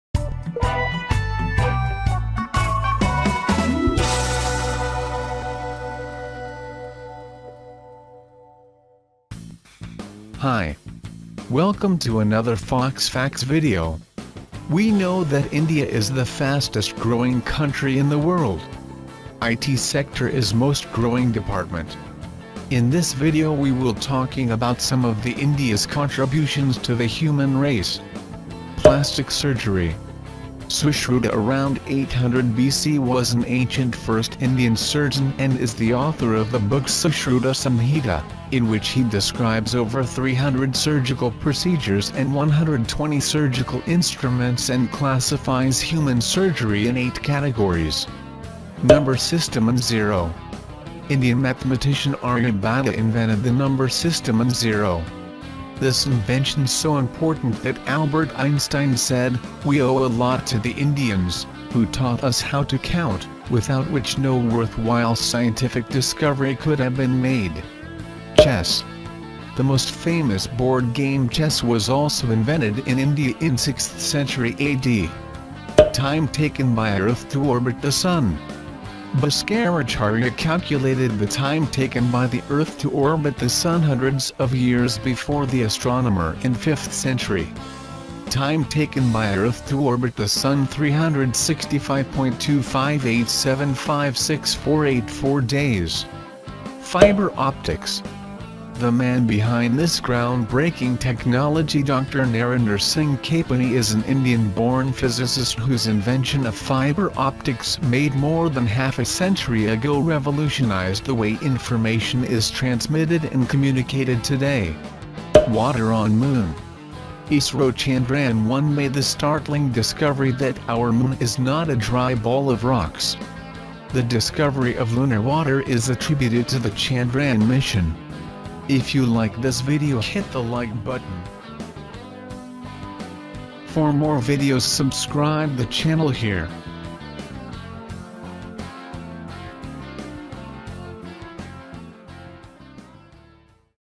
India’s Contribution to the Human Race { World }
We know that India is the fastest growing country in the world. IT sector is most growing Department.
In this video we talking about India’s contribution to the Human race.
# Plastic surgery.
# Zero and Number System.
# Chess.
# Time taken by Earth to orbit the sun.
# Fiber Optics.
# Water on Moon.